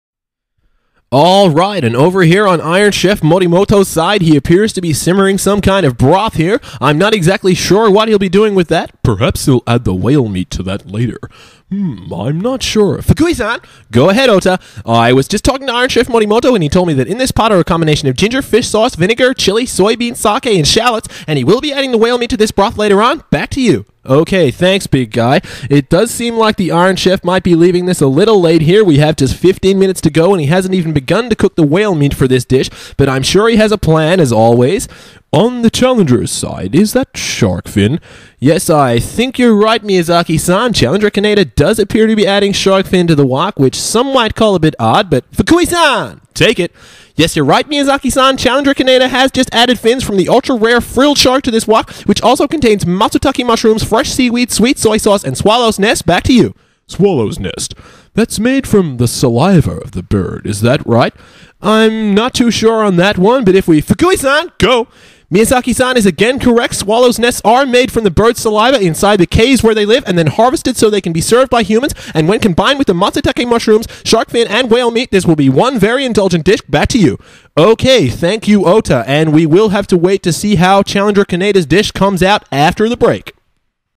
Iron Chef 1 (Cooking)
My impression of the cooking part of Ryouri no Tetsujin, or as it's known in English, Iron Chef.